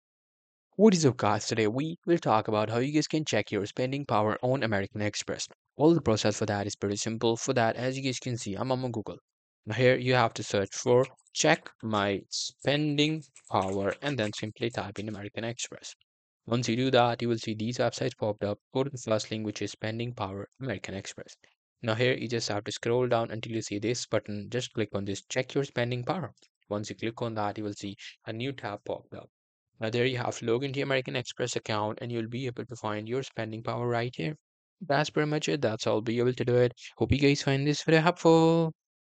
How To Check Spending Power AMEX 2024
In this video I will show you how to check spending power amex 2024